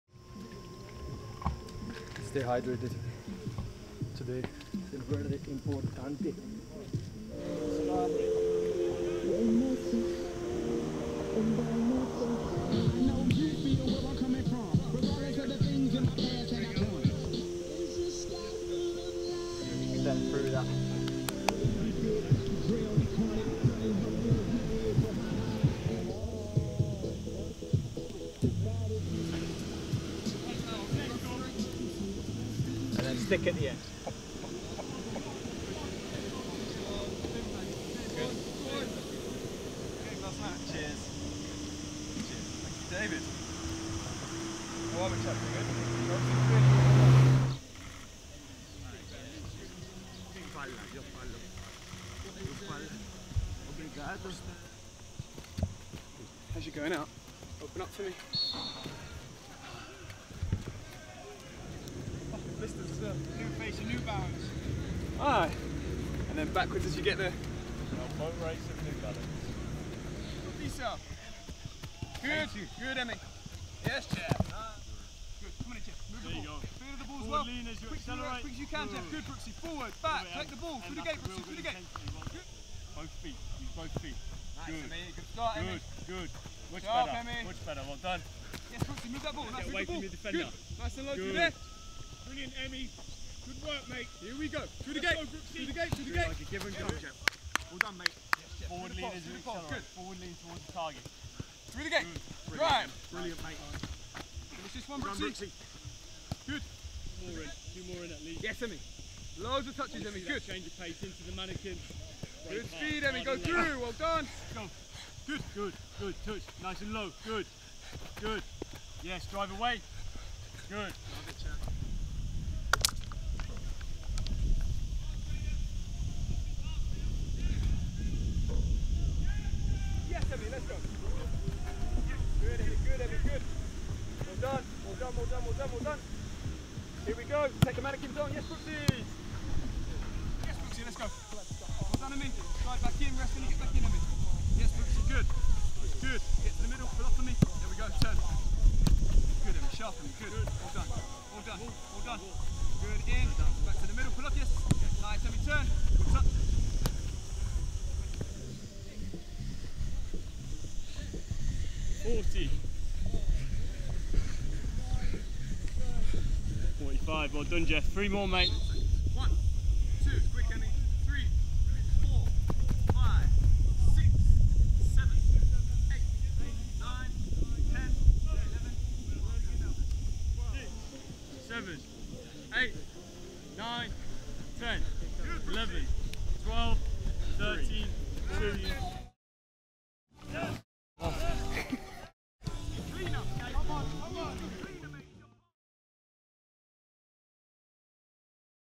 Lerma and Brooks join up with squad in Spain 🤝🇪🇸
Jefferson Lerma and David Brooks linked up with the Cherries squad in Marbella as the squad take in their first training sessions in Spain.